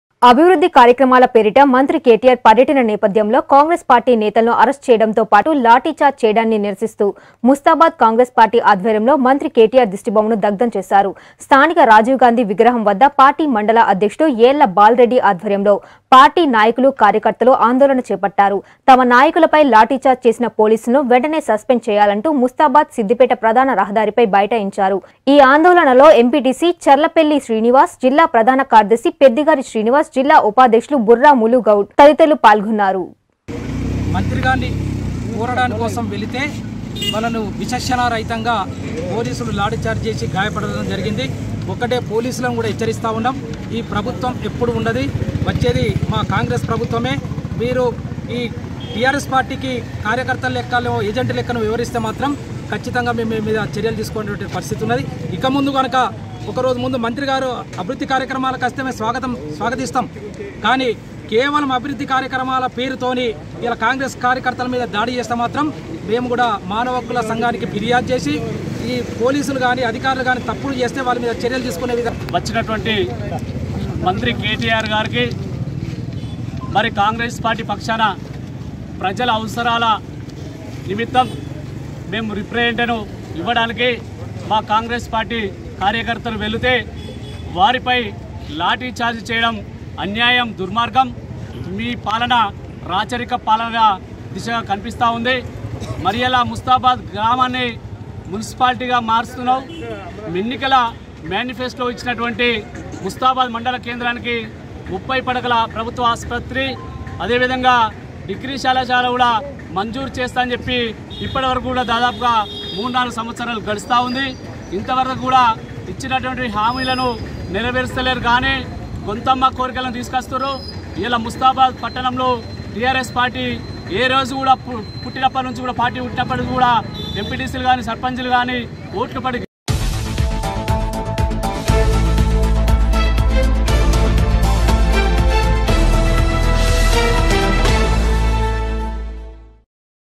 కేటీఆర్ దిష్టిబొమ్మను దగ్ధం చేసిన కాంగ్రెస్ నేతలు|b6news
అభివృద్ధి కార్యక్రమాల పేరిట మంత్రి కేటీఆర్ పర్యటన నేపధ్యంలో కాంగ్రెస్ పార్టీ నేతలను అరెస్టు చేయడంతో పాటు లాఠీచార్జ్ చేయడాన్ని నిరసిస్తూ... ముస్తాబాద్ కాంగ్రెస్ పార్టీ ఆధ్వర్యంలో మంత్రి కేటీఆర్ దిష్టిబొమ్మను దగ్ధం చేశారు.  స్థానిక రాజీవ్ గాంధీ విగ్రహం వద్ద పార్టీ మండల అధ్యక్షుడు ఏళ్ల బాల్ రెడ్డి ఆధ్వర్యంలో పార్టీ నాయకులు, కార్యకర్తలు ఆందోళన చేపట్టారు. తమ నాయకులపై లాఠీచార్జ్ చేసిన పోలీసులను వెంటనే సస్పెండ్ చేయాలంటూ ముస్తాబాద్-సిద్దిపేట ప్రధాన రహదారిపై బైఠాయించారు. ఈ ఆందోళనలో ఎంపీటీసీ చర్లపెల్లి  శ్రీనివాస్, జిల్లా ప్రధాన కార్యదర్శి పెద్దిగారి శ్రీనివాస్, జిల్లా ఉపాధ్యక్షులు బుర్ర ములుగౌడ్, తదితరులు పాల్గొన్నారు.